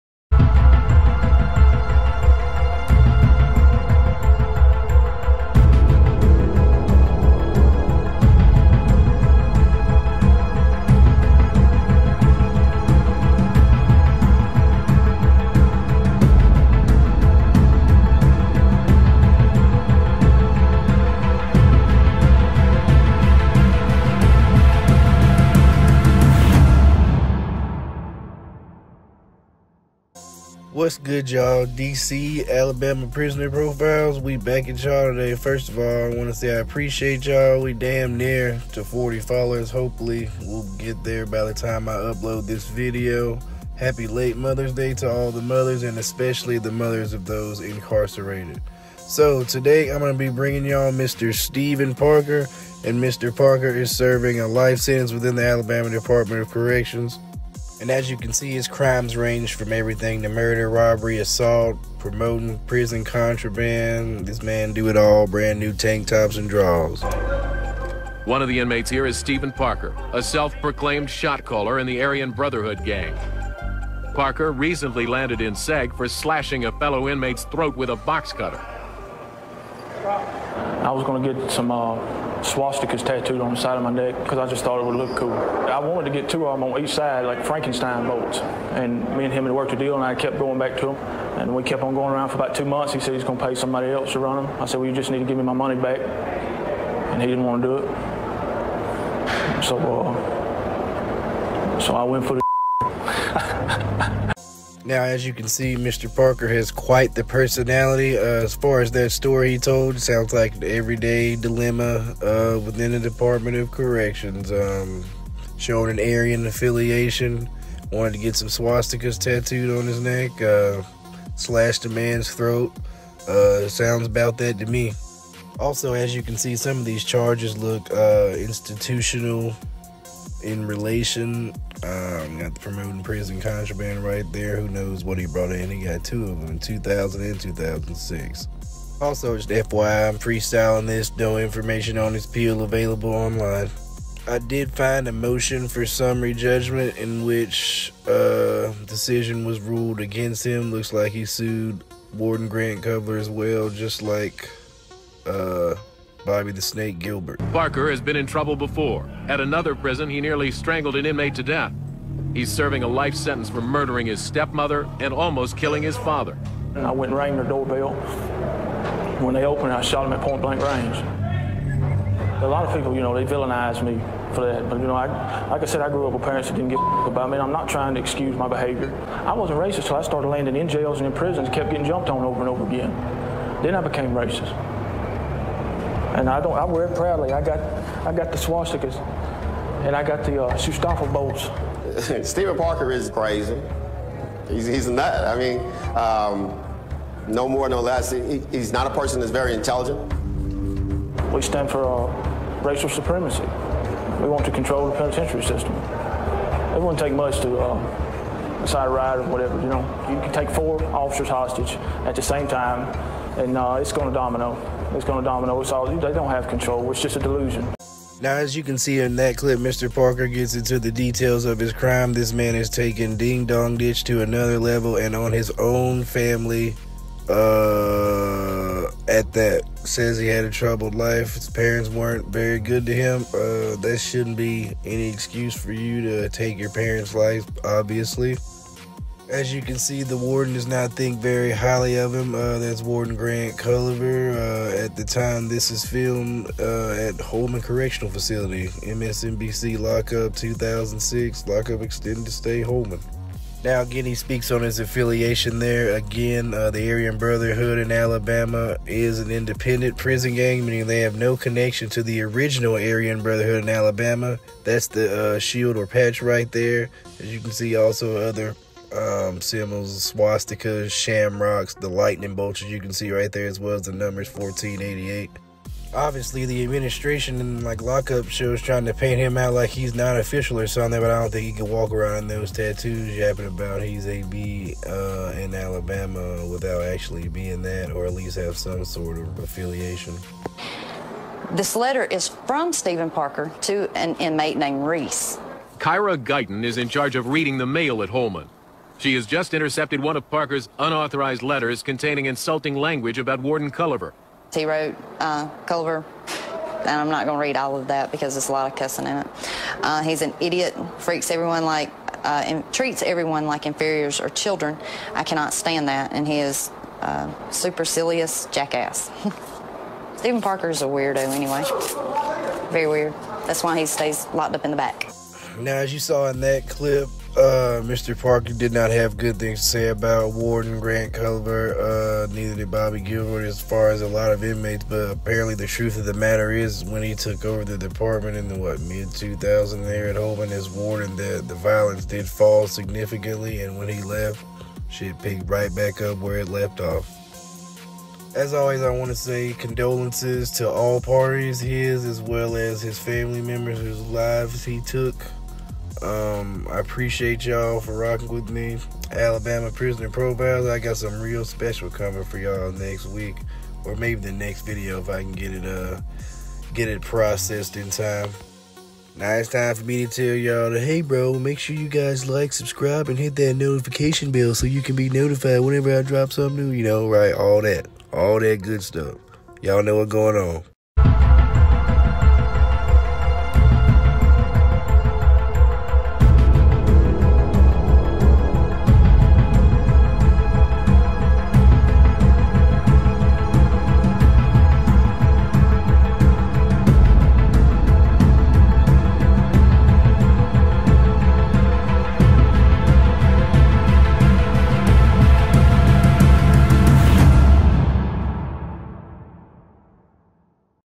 Alabama Aryans: Steven Parker
EP8 delves into the crimes and prison life of Aryan Brotherhood member Steven Parker. Imprisoned within the Alabama Department of Corrections serving LWOP.